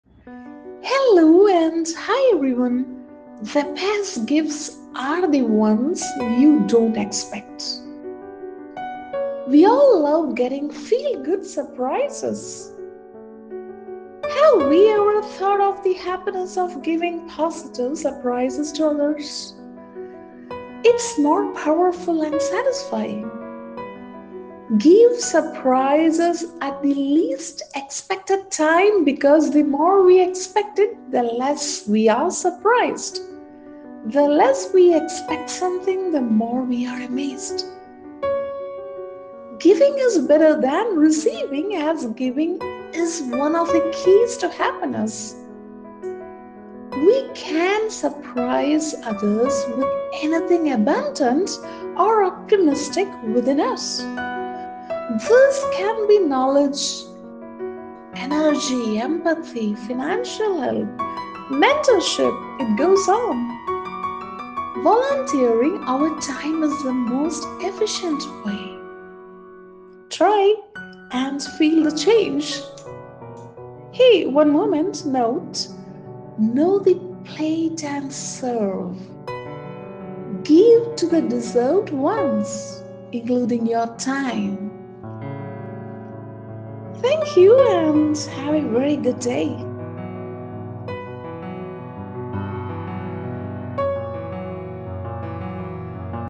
The best gifts are the ones you don't expect. Have you ever experienced the power of Giving?
Gurucharanam Saranam...The best gifts are the ones you don't expect. We all love getting feel-good surprises. Have we ever thought of the happiness of giving positive surprises to others? .. it's more powerful & satisfying. Give surprises at the least-expected time because the more we expect it, the less we're surprised. The less we expect something, the more we're amazed. Giving is better than receiving, as giving is one of the keys to happiness.